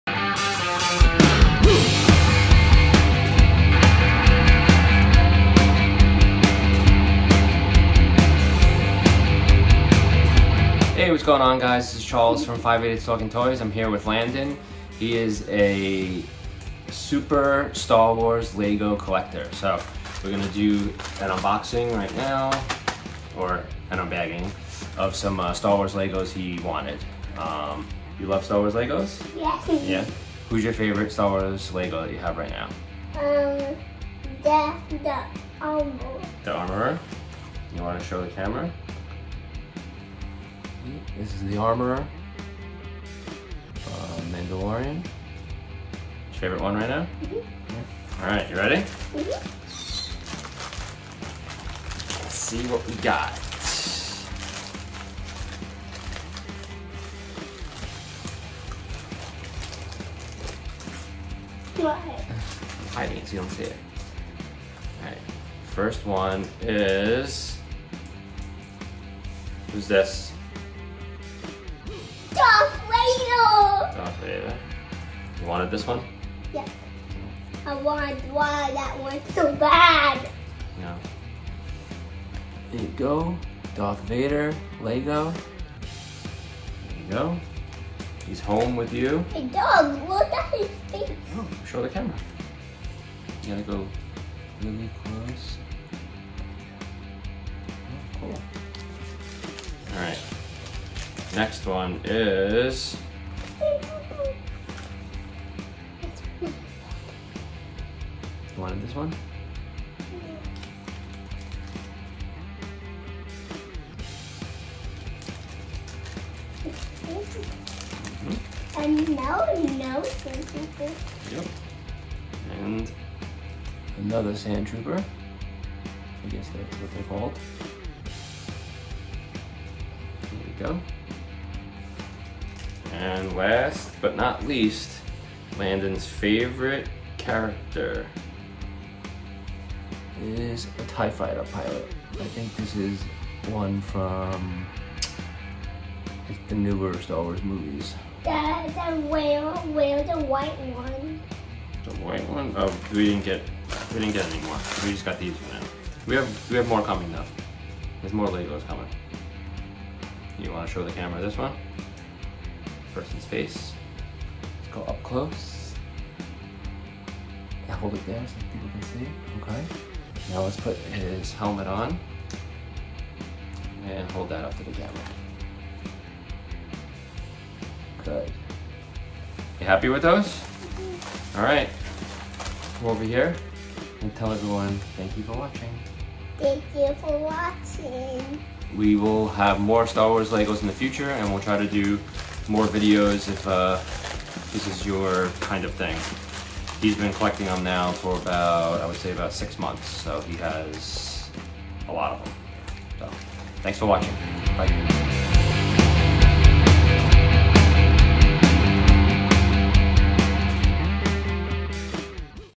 Father and Son Unboxing! | Charles and Landon Unbox LEGOS!!
Join Charles and Landon as they unbox some AWESOME Legos to add to Landon's collection!!

Legos were purchased from Hollyhock Toy Shop  Check them out on ebay